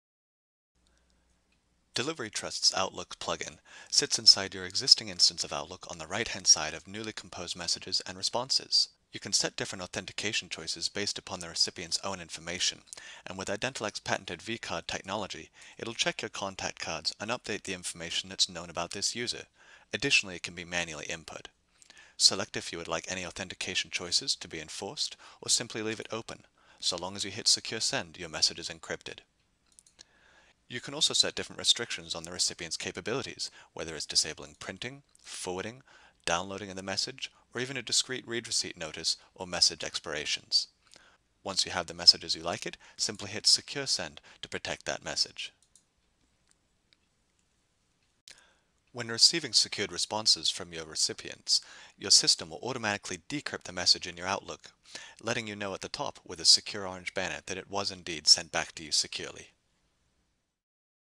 Identillect - Delivery Trust - Outlook Plugin
Seamlessly send and receive encrypted emails in Outlook 2010/2013/2016 for PC using our convenient Outlook add-in.